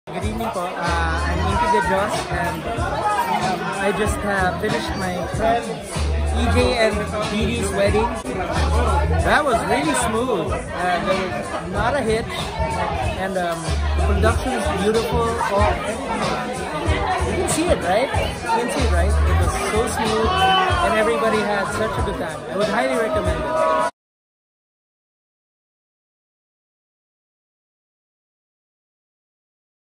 8willhappen Client Feedback - Wedding Guest - Ej and DD’s Wedding
Wedding Coordination by 8willhappen Events Management

Venue: Almond Garden Tagaytay